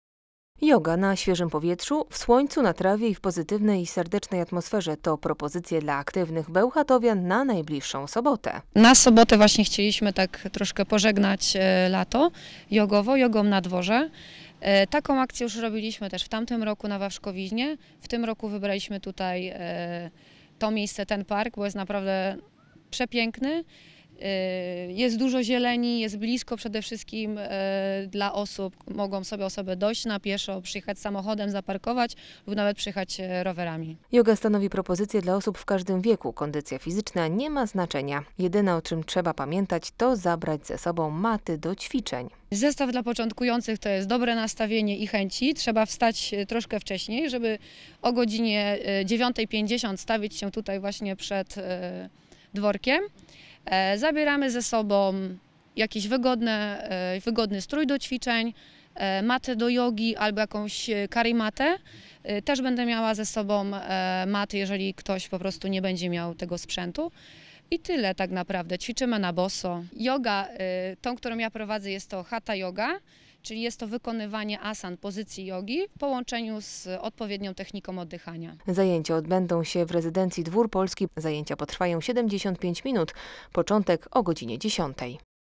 TKB - Joga w parku - 25.08.2016
Przed nami ostatni wakacyjny weekend, który nie musi oznaczać błogiego lenistwa. Mamy propozycję dla aktywnych bełchatowian – którzy chcą zadbać o swoją kondycję na świeżym powietrzu.

Joga w parku to propozycja dla aktywnych. Zajęcia są bezpłatne i odbędą się w Rezydencji Dwór Polski przy ulicy Czaplinieckiej 177. Zajęcia potrwają 75 minut - początek o godzinie 10:00